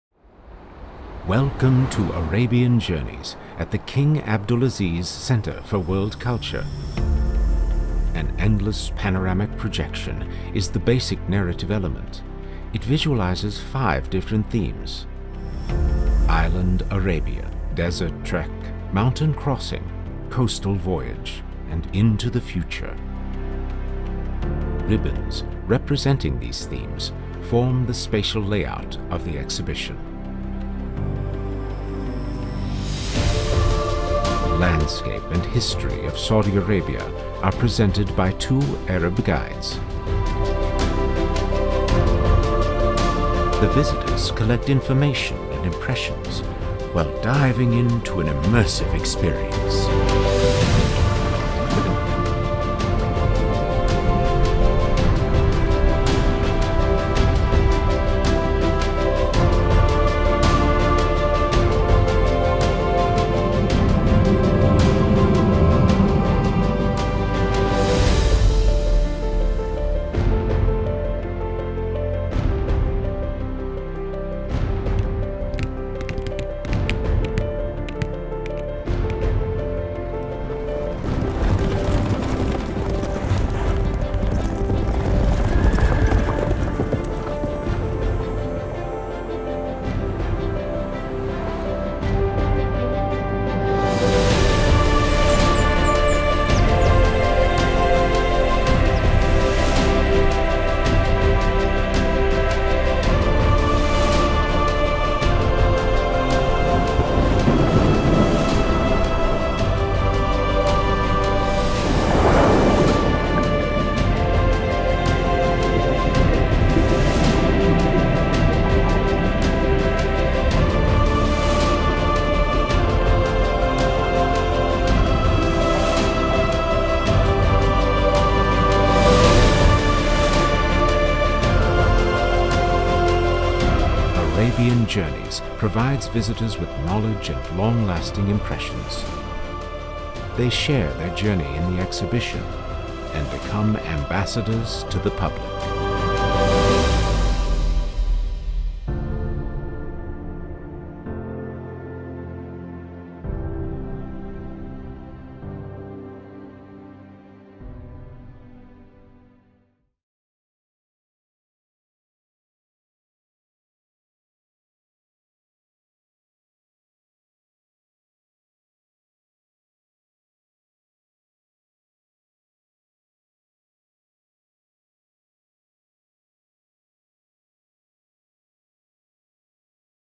ARABIAN JOURNEYS Dhahran, King Abdulaziz Center For World Culture
"Arabian Journeys" presents the roots and cultural identity of Saudi Arabia. The exhibition consists of a multi-media journey through the Arabian country's past, present and future. An extended panoramic strip demarcates individual exhibition areas; each with its own thematically arranged focus. Impressively staged narrative spaces engulf the visitor in specific landscape formations while offering access to the local people and their multi-facetted histories.

General Planning, Exhibition Concept and Design, Scenography: ATELIER BRÜCKNER
Light Planning with: LDE Belzner Holmes
Media Planning with: medienprojekt p2
Media Design with: iart
Graphic Design with: Tarek Atrissi Design
Film Production: iart with TAMSCHICK MEDIA+SPACE
Sound Design: iart with idee und klang
Architecture: Snøhetta
Photography: Daniel Stauch